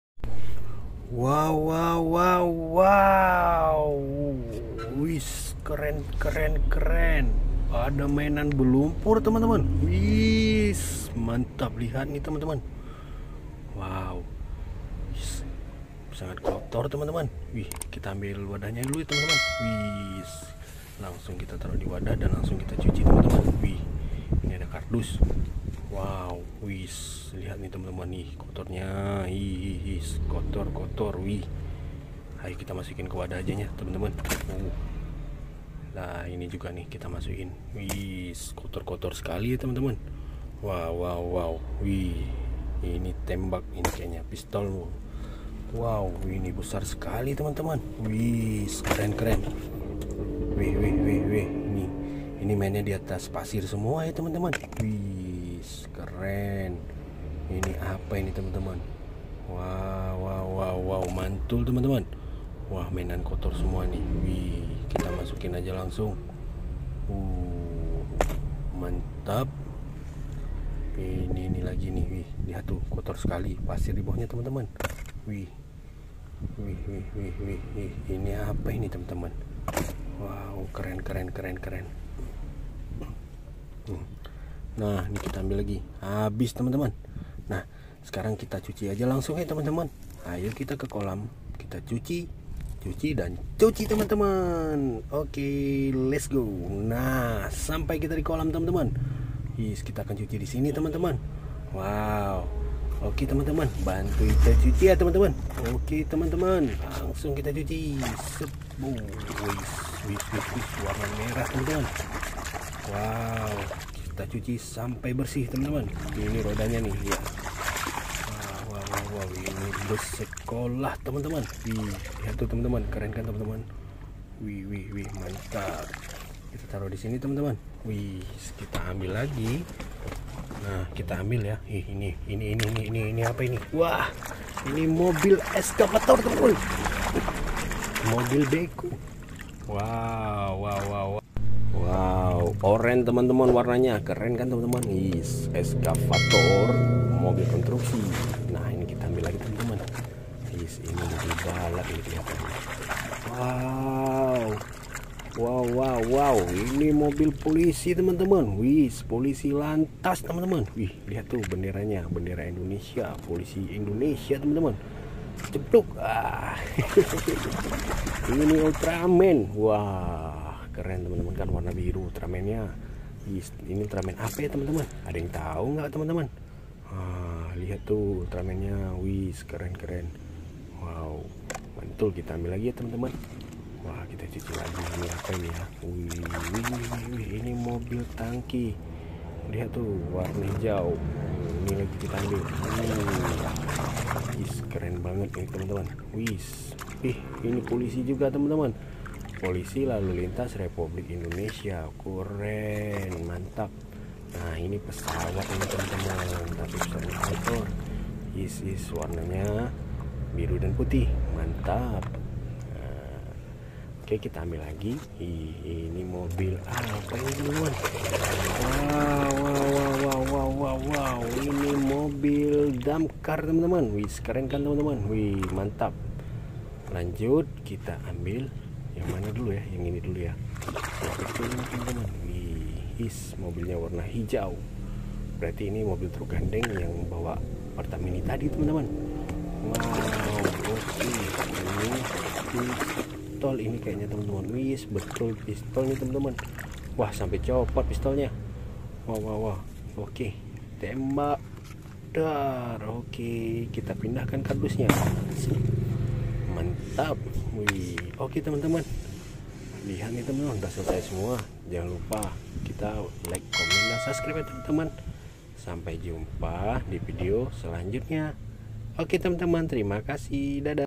Membersihkan Mainan Mobil Mobilan Excavator, Mobil Polisi Biru, Pesawat Terbang, Mobil Bus Sekolah
Membersihkan Mainan Mobil Mobilan Excavator, Mobil Polisi Biru, Pesawat Terbang, Mobil Bus Sekolah, Mobil Damkar, Ultraman zero, Truck Gandeng Tangki Pertamini, Spiner.